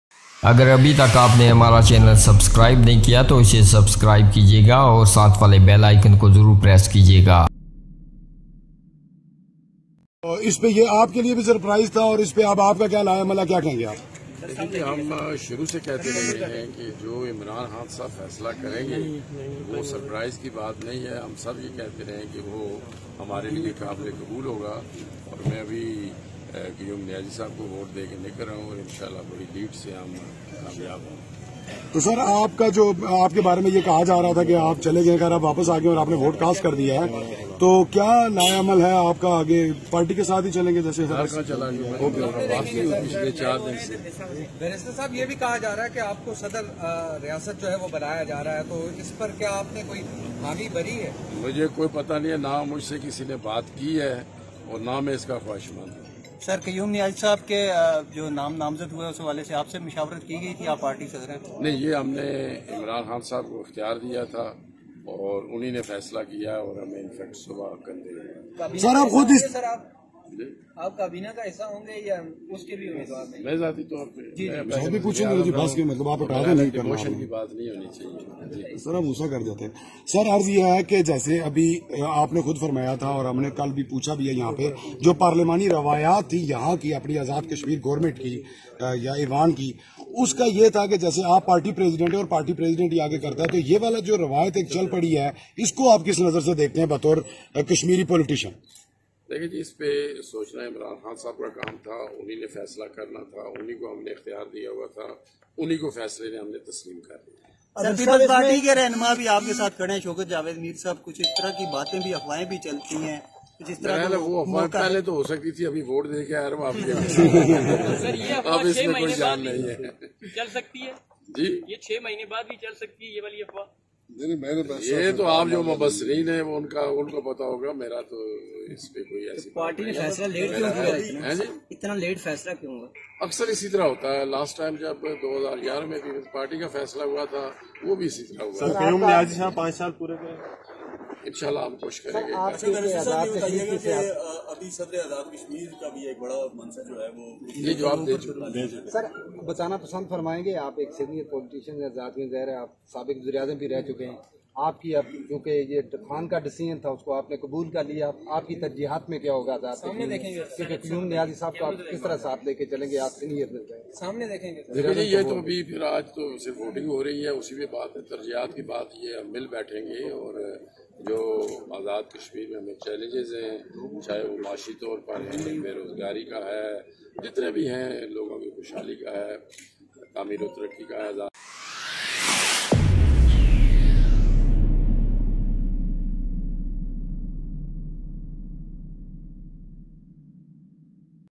Barrister Sultan Mahmood Chaudhry talks to media || 4 AUG 2021 || IrajStarTv
About this Video:
kotli azad kashmir news, azad kashmir news live, dadyal azad kashmir news,
azad kashmir news rawalakot, daily ajk news, ajk news live today,
mirpur azad kashmir news today, ajk news today,